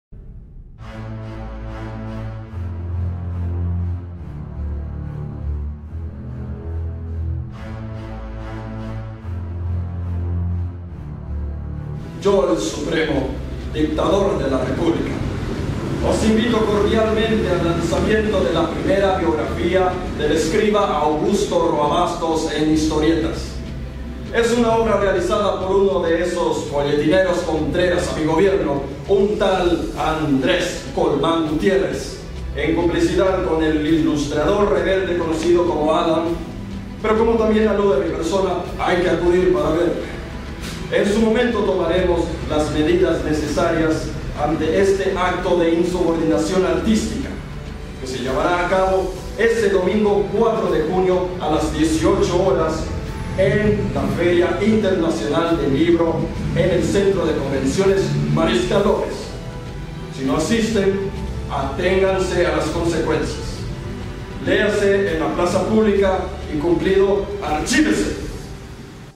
EL SUPREMO OS INVITA...
El Supremo, personaje literario de la novela "Yo El Supremo", "invita cordialmente" al acto de presentación del álbum "Augusto Roa Bastos, el supremo escritor", biografía del autor en historietas, con guión de Andrés Colmán Gutierrez, dibujos de Danie Ayala Medina (ADAM). La cita es el domingo 4 de junio de 2017, a las 18, en el Auditorio José María Rivarola Matto, de la Feria Internacional del Libro de Asunción, Centro de Convenciones Mariscal López. Es parte de la colección "Protagonistas de la Historia en Paraguay", del sello ServiComic, de la Editorial Servilibro. ¡Les esperamos...!